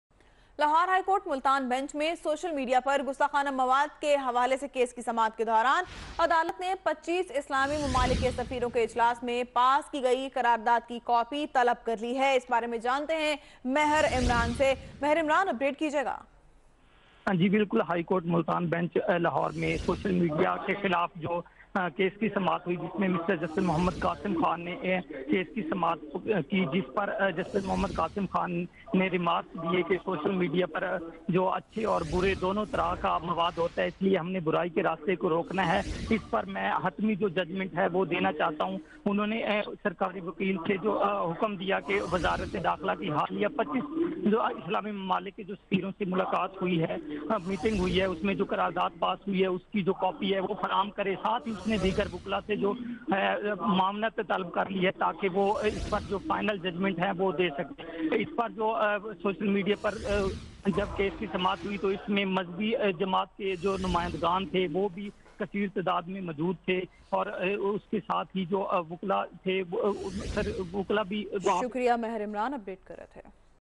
Lahore High Court Multan bench hearing in case of blasphemous content on social media
24 News HD is one of the leading news channels of Pakistan bringing you the latest current affairs from Pakistan and around the world.